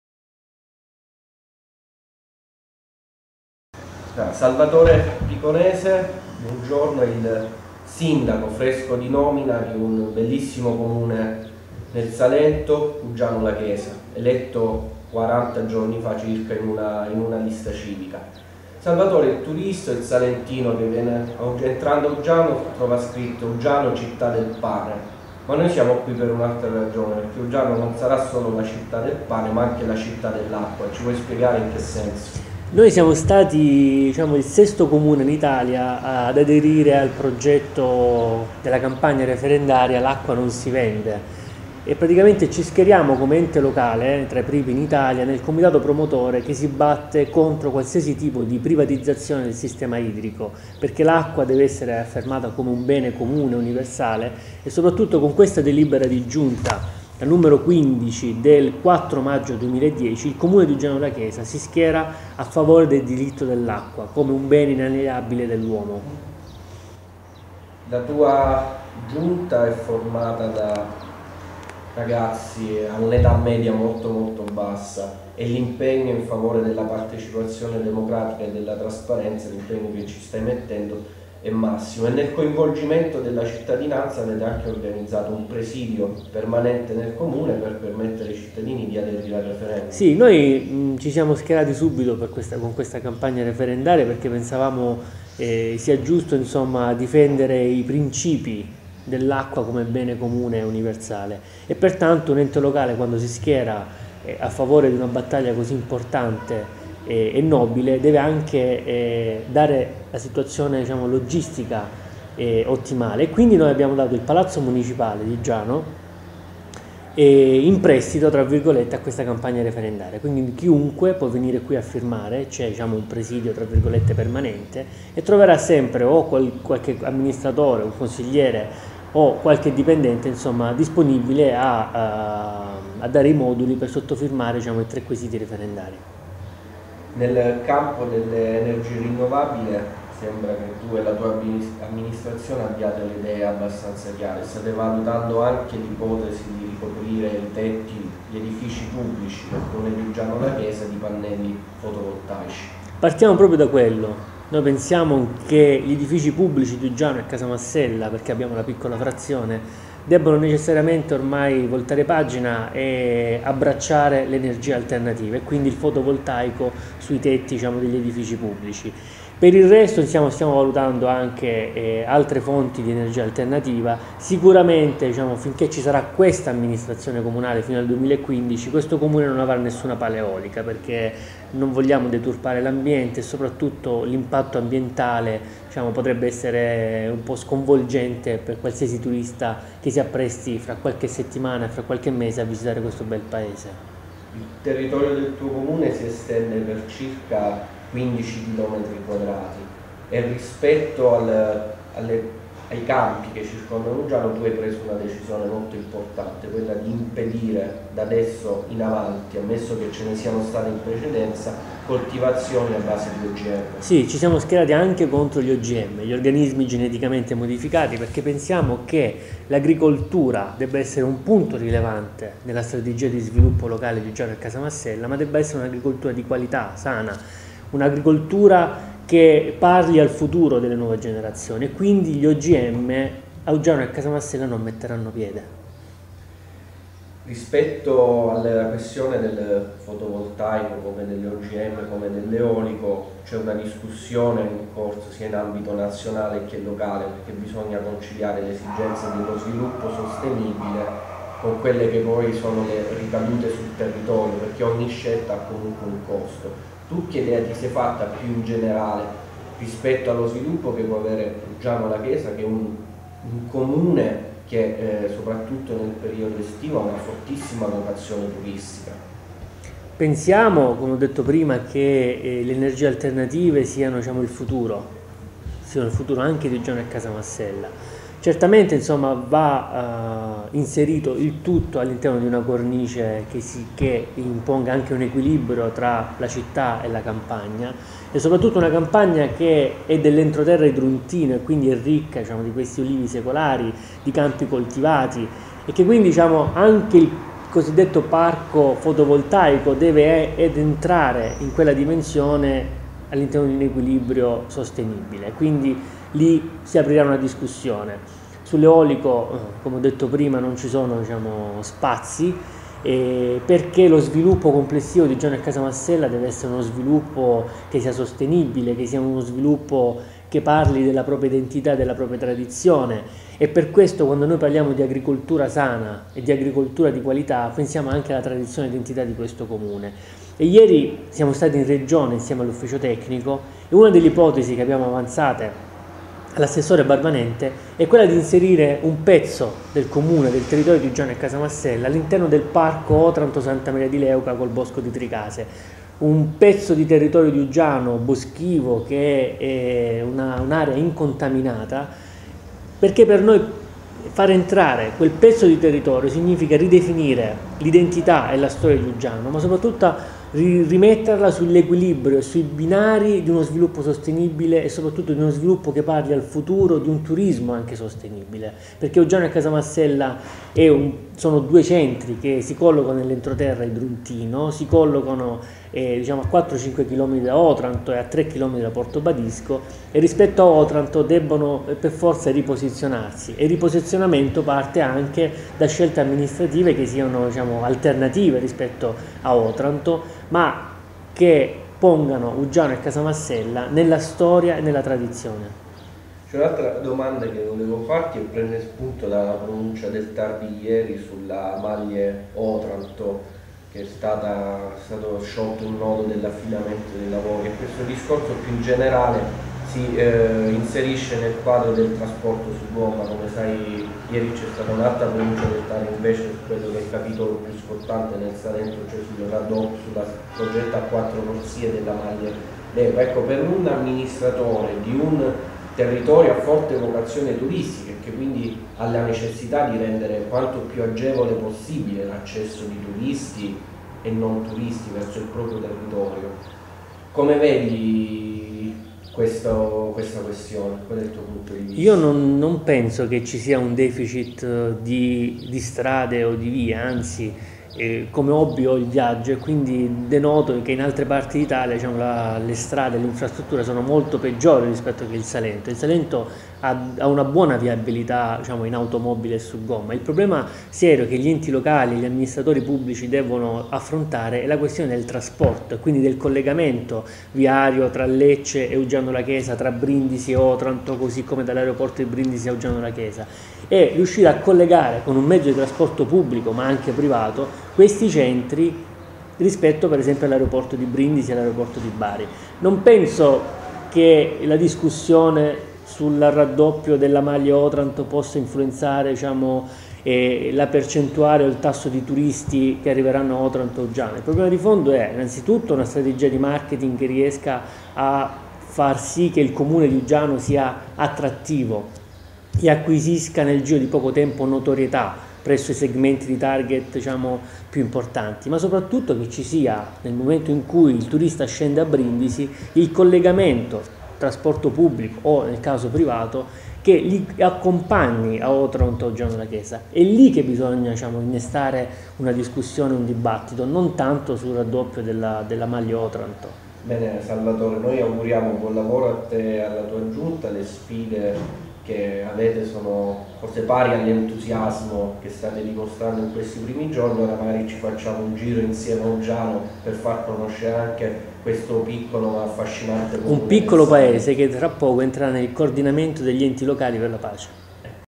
Uggiano La Chiesa, lo sviluppo è sostenibile
Insediato da poche settimane come sindaco, Salvatore Piconese ha le idee chiare sulle direttrici di sviluppo per Uggiano La Chiesa, comune
salentino a pochi chilometri da Otranto. Difesa dell'acqua come bene pubblico, divieto di coltivazioni OGM, inclusione nei progetti di valorizzazione turistico paesaggistica come il parco Otranto Leuca o quello archeologico di Porto Badisco, adesione alle associazioni degli enti locali per far pesare di più la propria voce. Nel video l'intervista di Gabriele De Giorgi.